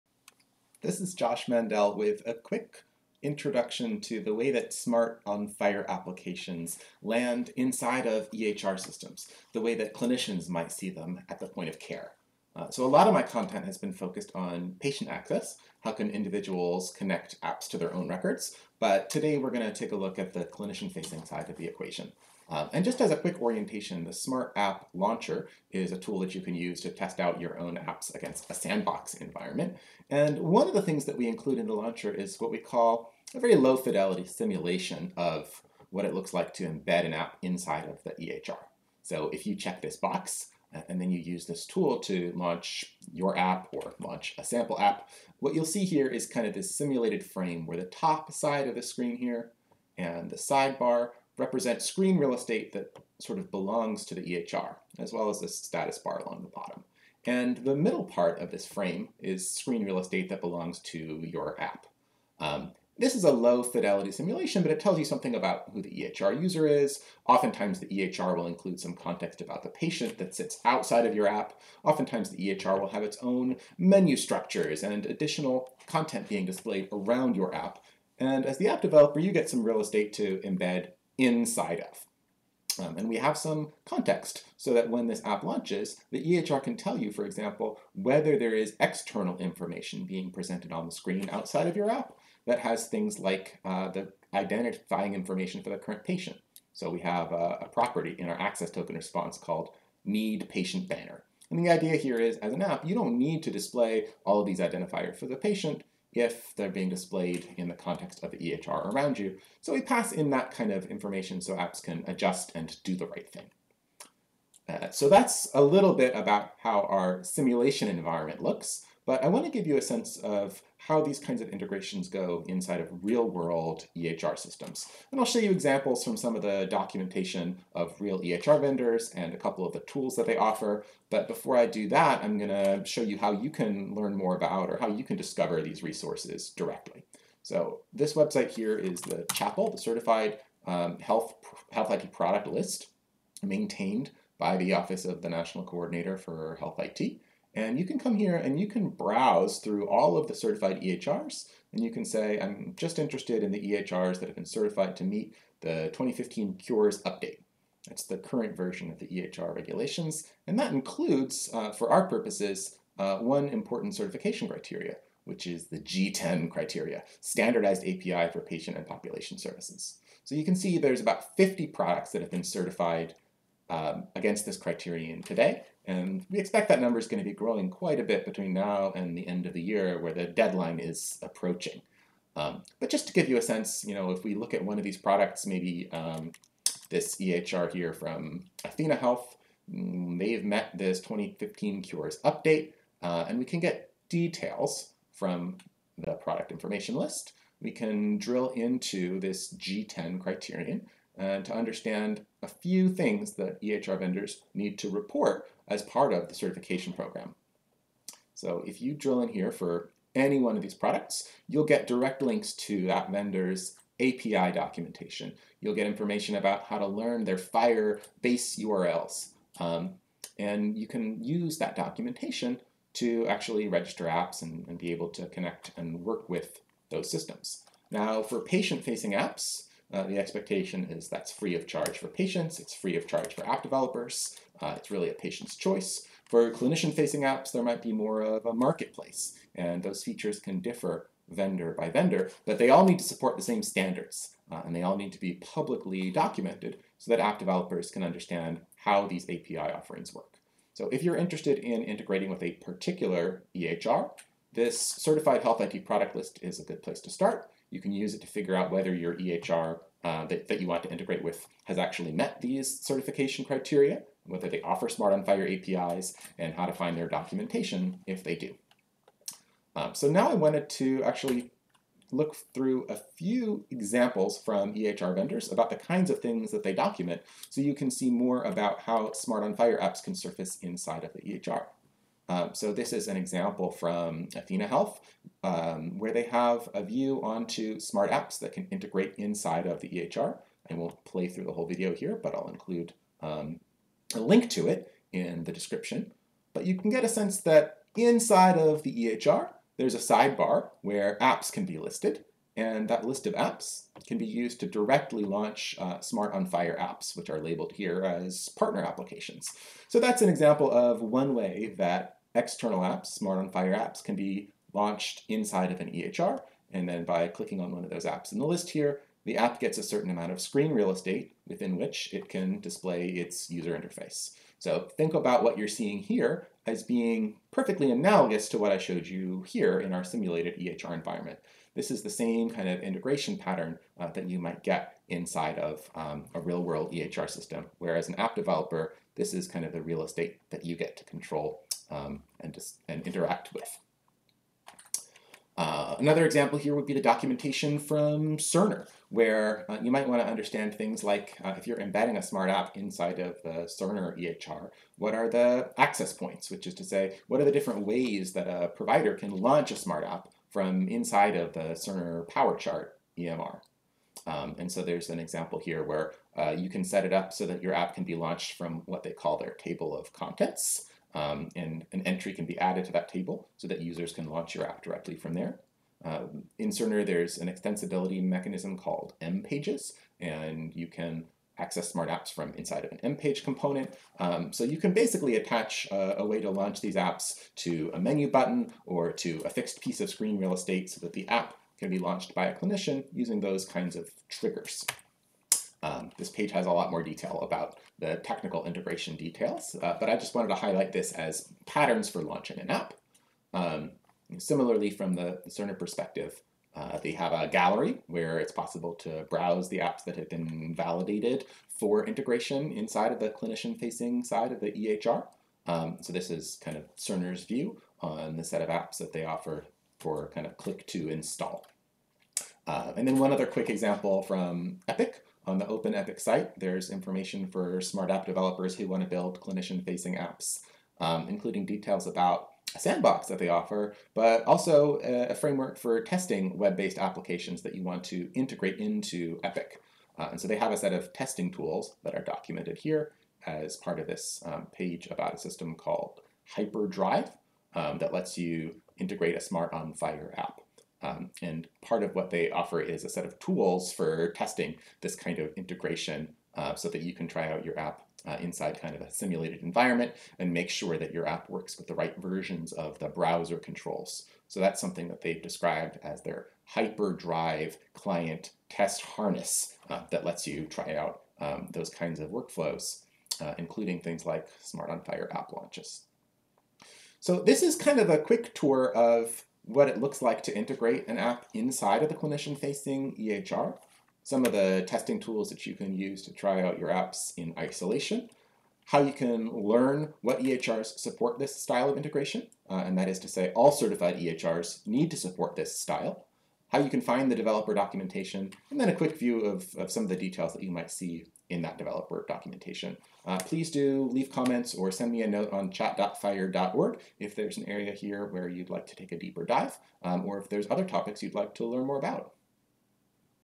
SMART App Integration in EHRs
In this video we look at the UI aspects of integrating SMART on FHIR Apps into clinician-facing EHRs. We review the basics using the open-source SMART App Launcher, and then dig into ONC's official "Certified Health IT Product List" to see how to browse an authoritative list of certified EHRs, find their developer documentation, and get started integrating SMART on FHIR Apps. We also browse through example documentation from a few vendors to review the kinds of guidance they provide for integration of clinician-facing apps.